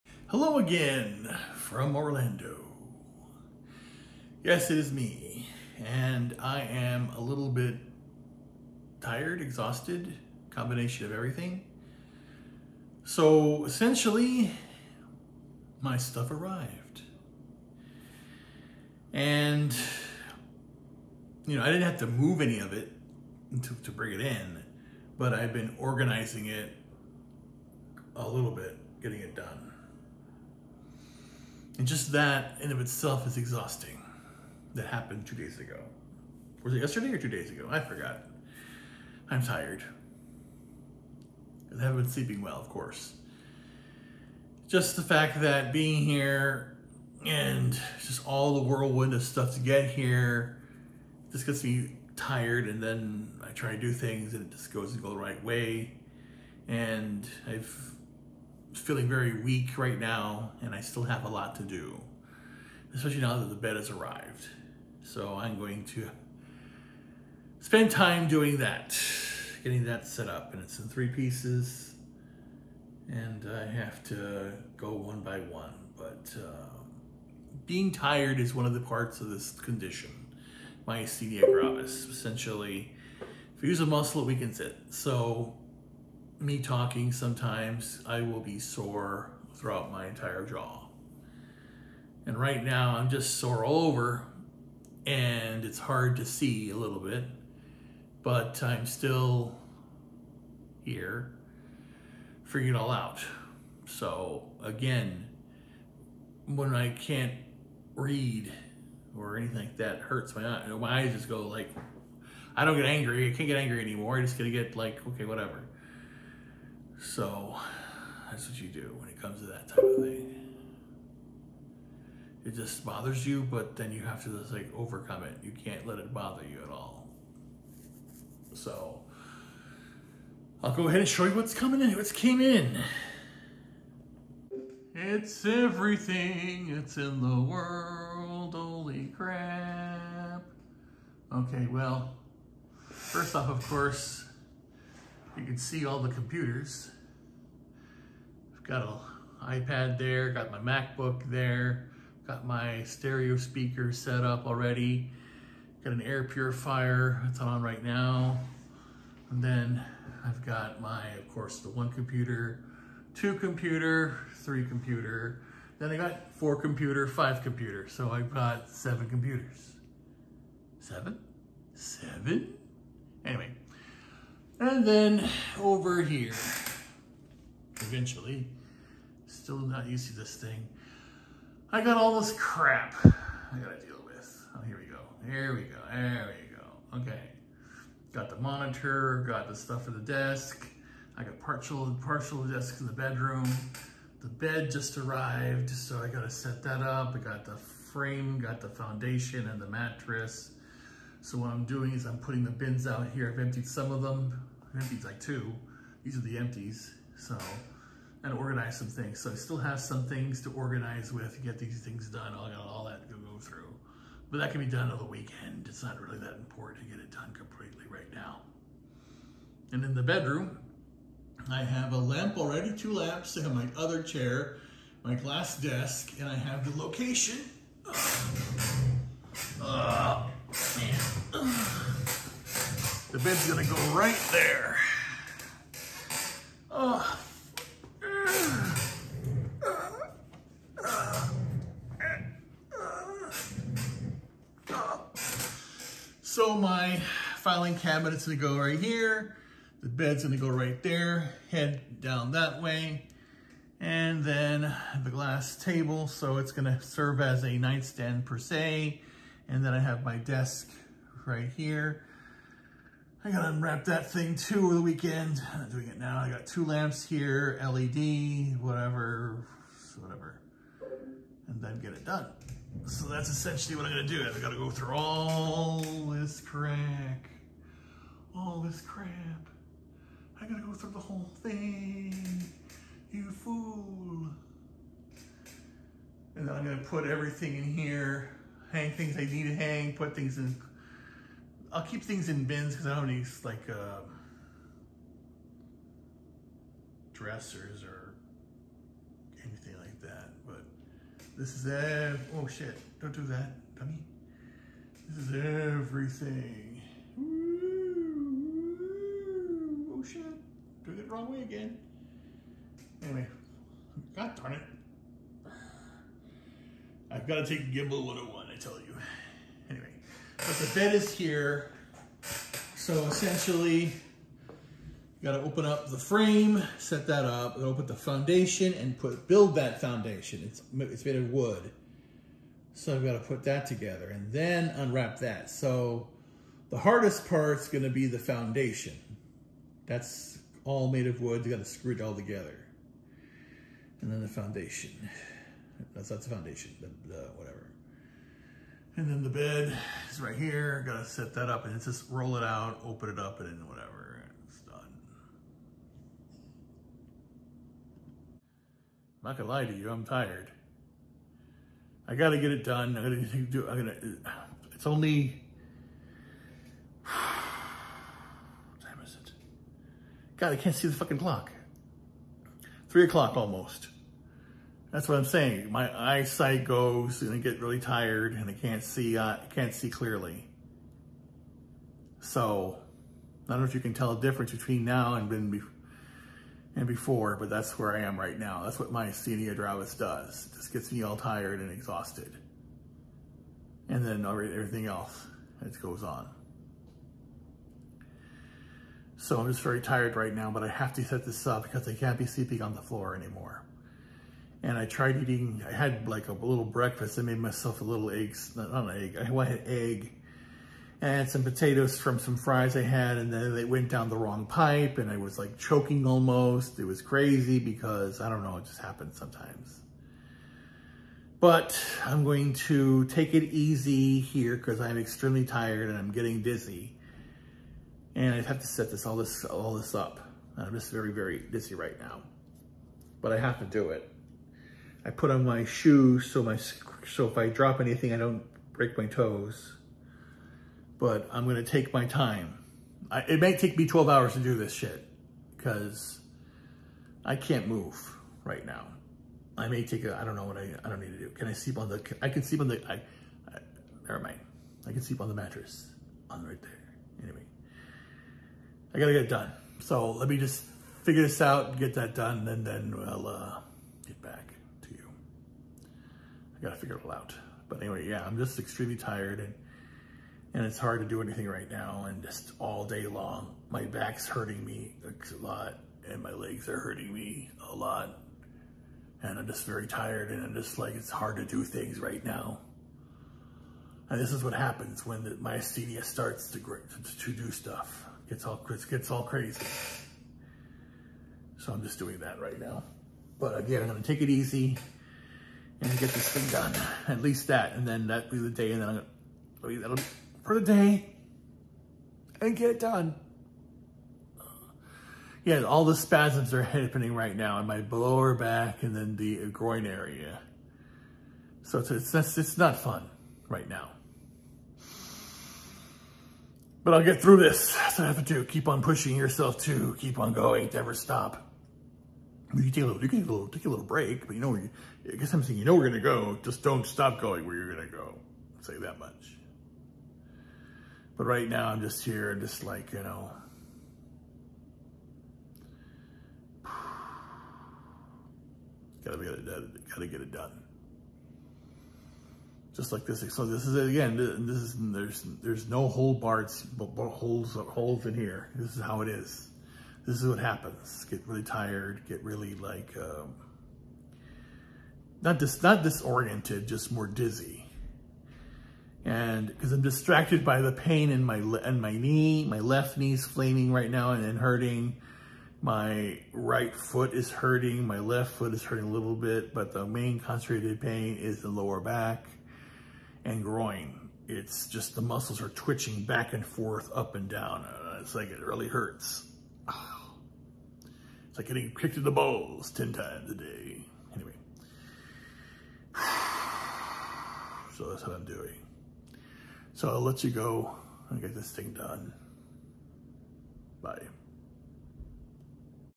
The first wave of my belongings as arrived! Yet I am in extreme pain and exhaustion.
My myasthenia gravis is acting up and I am extremely exhausted and every muscle in my body is hurting and in spasm.

But at least I got my bed today. Now I need to set it up oh joy￼.